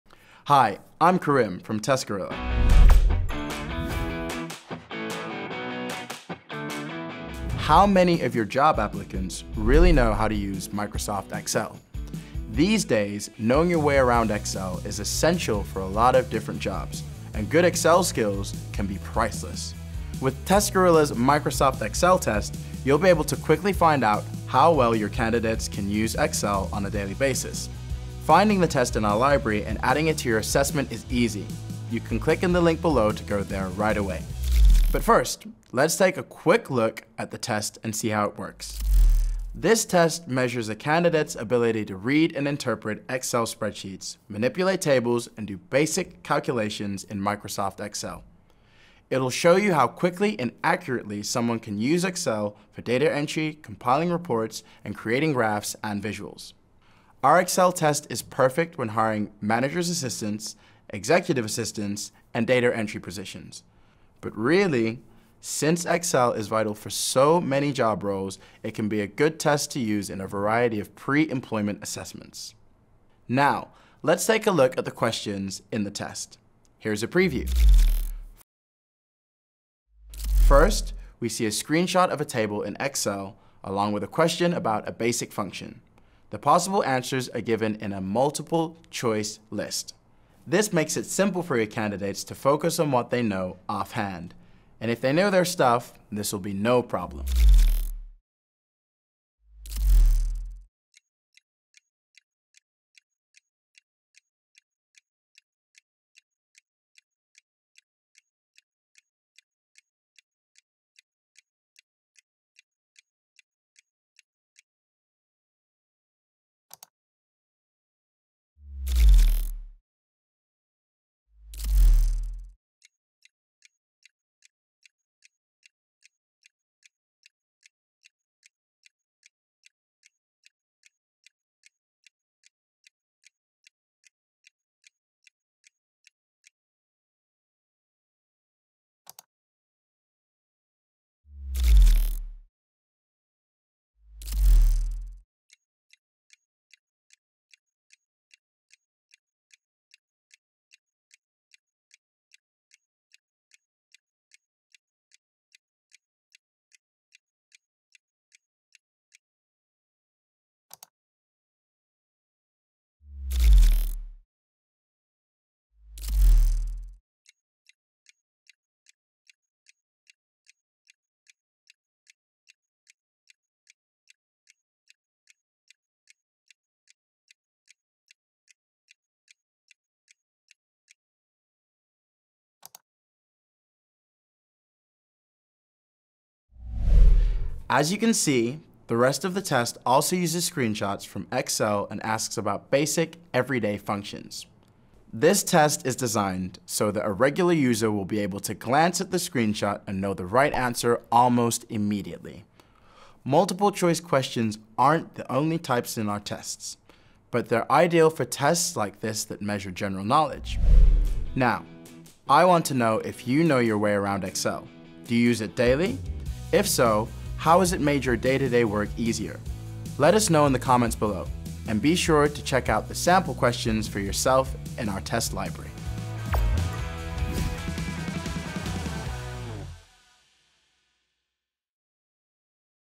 Hire the best with TestGorilla's Microsoft Excel test
Let the TestGorilla Microsoft Excel Test help you understand your candidates' abilities without bias. Our Microsoft Excel Test can help streamline your hiring process and give you insight into which candidates are technically best suited to the position.

When recruiting, it is crucial to identify the best candidate and accurately predict their future performance and prospects of success. By adding a pre-screening assessment to your recruitment process, you can review several candidates and compare their capabilities before the interview process.

Skill-specific pre-screening tests can help recruiters like you understand candidate abilities and competencies without bias. Adding an Excel test to your pre-screening assessment can help you review several candidates and compare their skills before you start interviewing. Additionally, basic Excel testing can streamline the hiring process and allows you to see which candidates are technically best suited to the position.

TestGorilla's test library offers two different Microsoft Excel tests for recruiters to review candidates on different skill levels, depending on role requirements:

The general test helps you assess a candidate's ability to interpret and manipulate spreadsheets, perform basic calculations, and develop tables as part of Microsoft Office.
The advanced test helps determine whether candidates can master a range of Excel functions and formulas. This test gives you insight into how well candidates can organize and interpret data in Excel.

Head over to the TestGorilla to take a look at our tests and sign up